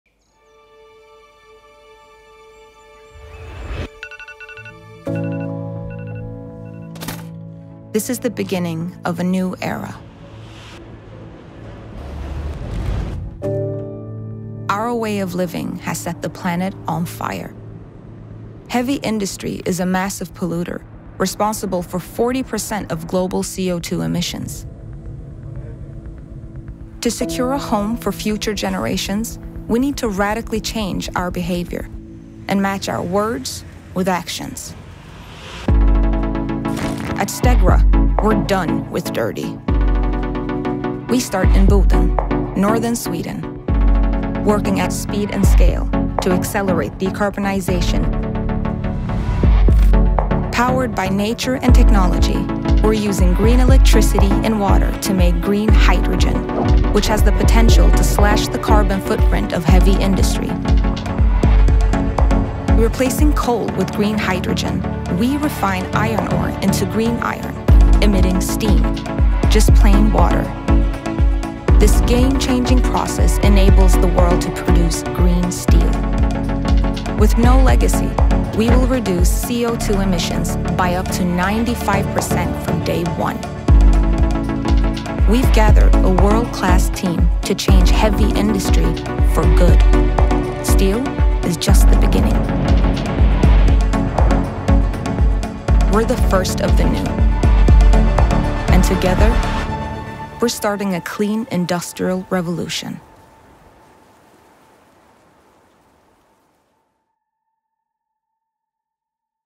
We are Stegra – the first of the new
Founded in 2020, Stegra was created with the purpose to decarbonize hard-to-abate industry. Construction of our first project – an integrated plant producing green hydrogen, green iron and green steel – is well underway in Boden, Sweden. Slashing emissions using green hydrogen and renewable electricity, we’re starting a clean industrial revolution. Steel is just the beginning.